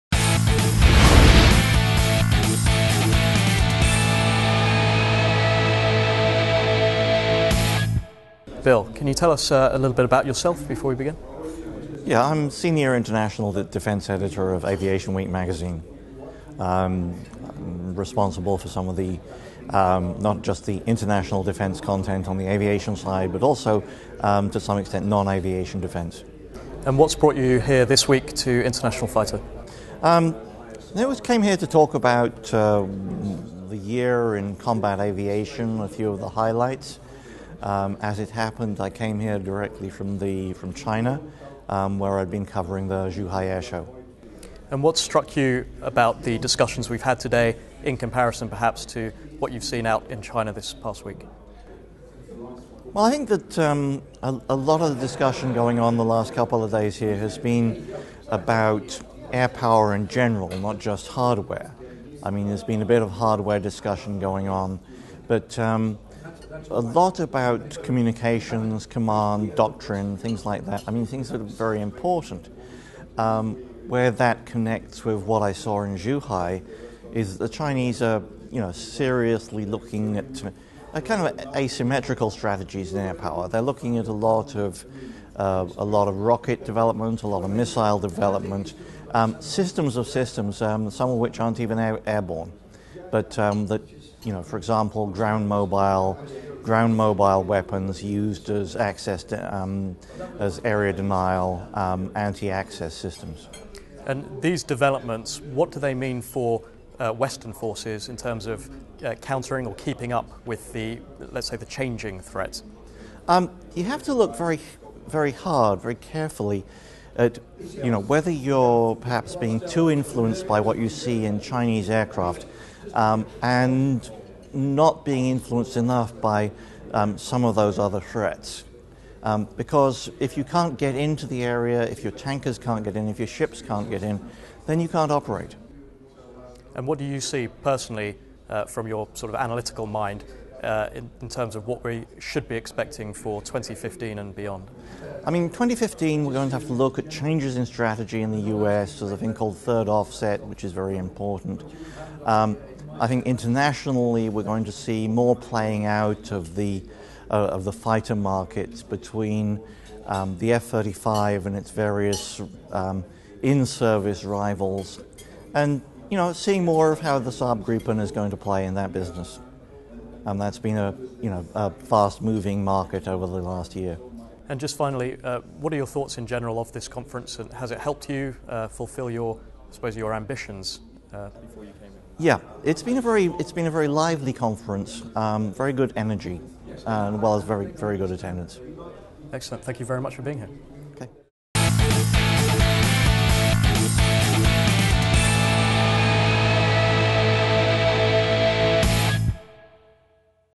Bill Sweetman, Aviation Week: International Fighter
Bill Sweetman, renowned military aviation expert and Senior International Defense Editor of Aviation Week & Space Technology, provides a brief overview of the international fighter domain, including the introduction of the F-35 and the technology recently revealed in China.